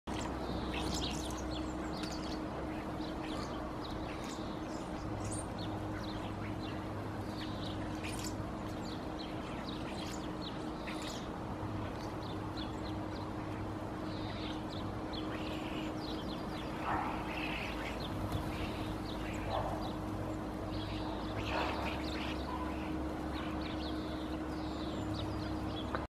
Starlings foraging on the "food aid site", vocalizing
Flock of Common Starlings (Sturnus vulgaris) foraging on the "food aid site", vocalizing.

Kottaraisparvi (Sturnus vulgaris) nauttii "ruoka-avusta"; ääntelyä.